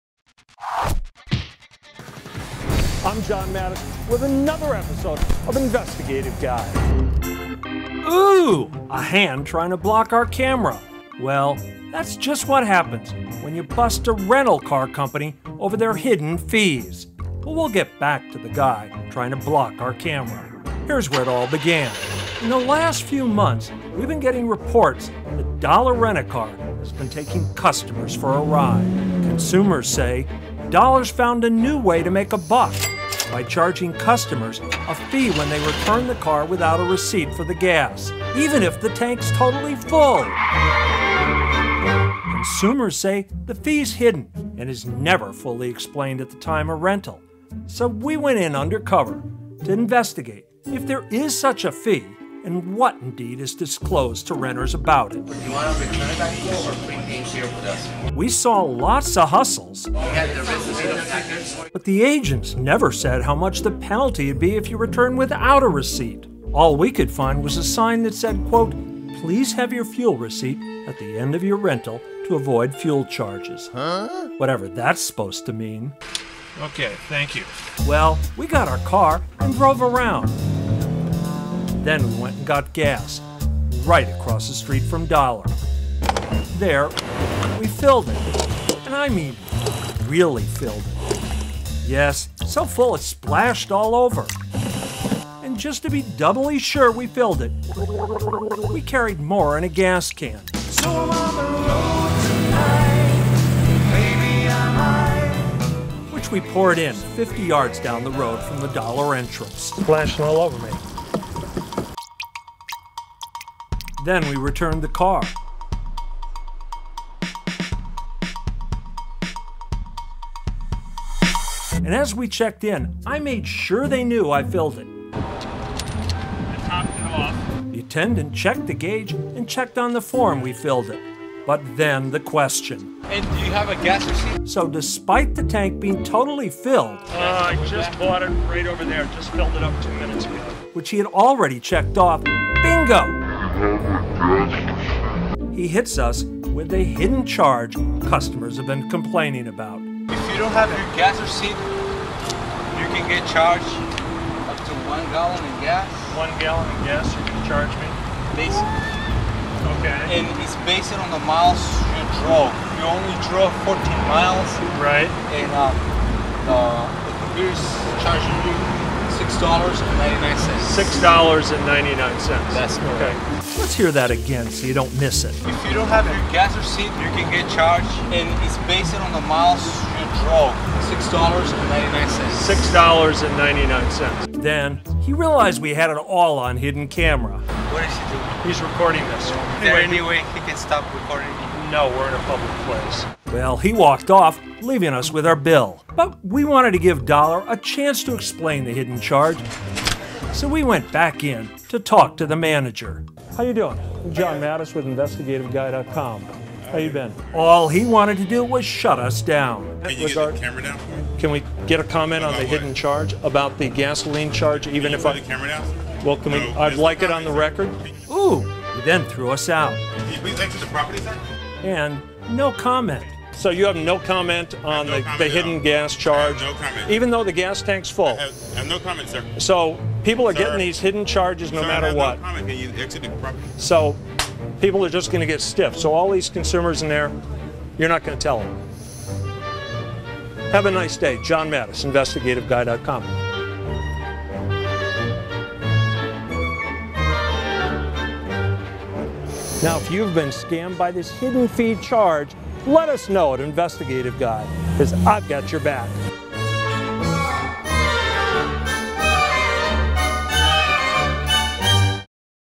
Dollar/ Thrifty Rent A Car Hidden Fees
Everyone knows the deal where car rental companies charge you outrageous fees to re fill the car if you bring it back  so much as one pint under full. Well Dollar Rent A Car has a  new hidden fee  on top of the standard ones  regarding filling the tank. They charge you a  "fuel charge " if you bring the car back FULL but don't have a receipt. Yes, you heard right they hit you with  a refueling charge even if you brought it back full. The charge is hidden they never tell you the amount of the "fuel charge" till you check out. So if you forgot a gas  receipt  get ready to get  to get hit with a made up charge when you check in. Sounds outrageous so we   went in undercover to find out.

That is not all,subesquently and unrelated now consumers allege they are being charged for insurance even after they decline it. If this has happened to you go for more information.